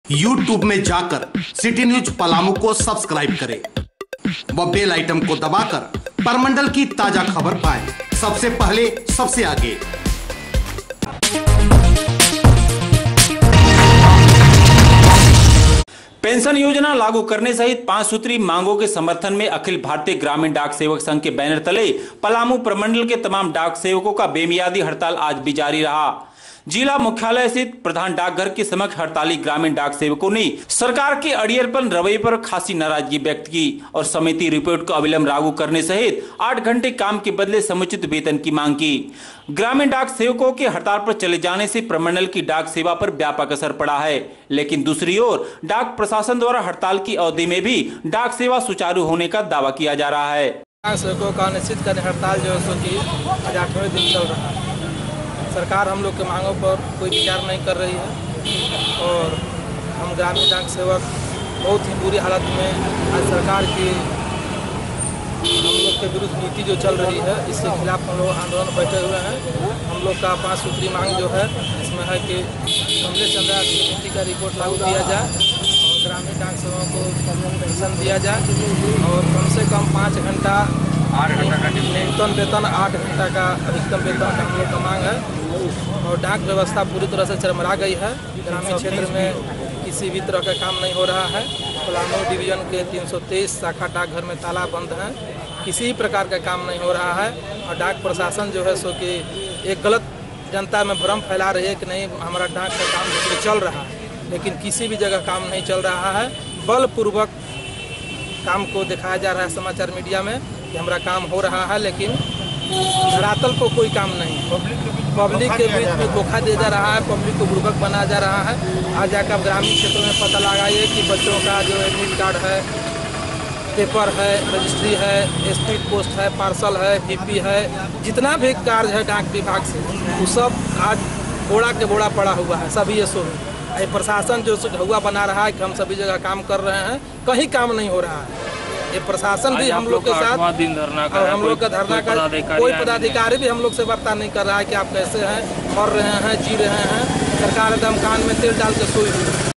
पलामू प्रमंडल की डाक व्यस्था चरमरायी ,ग्रामीण डाक सेवक संघ का हड़ताल जारी
मांगे पूरी नहीं होने तक आंदोलन जारी रखने की कही बात